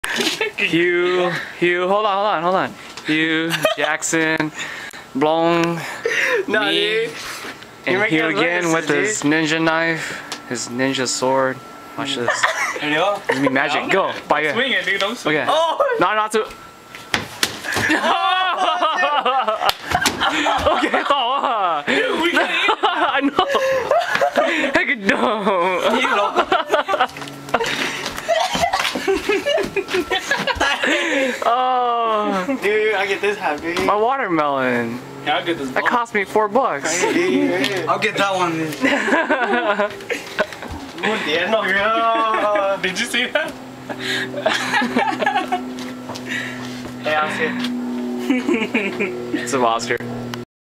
Our Watermelon Chop
This is dorm life at Simpson University. Fall 2008. Phillip, Blong, Jackson, and Hue.